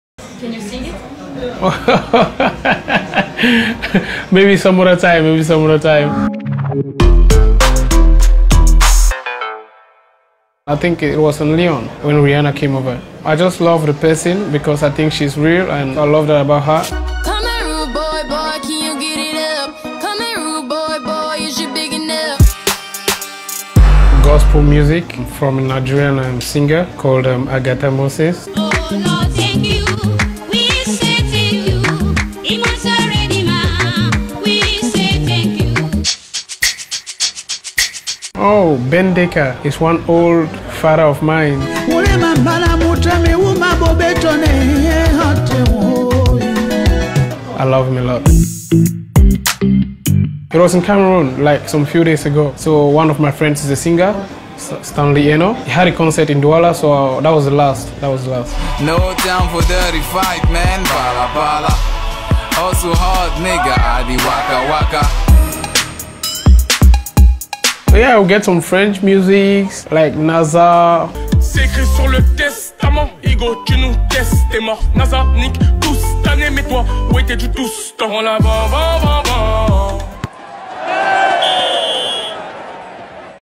Clinton Njie est fan de Rihanna, voici sa playlist foot
"Mon premier concert ? C'était Rihanna à Lyon. Je l'aime beaucoup, parce que je pense qu'elle est vraie."

Découvrez la playlist foot de Clinton Njie de l'Olympique de Marseille !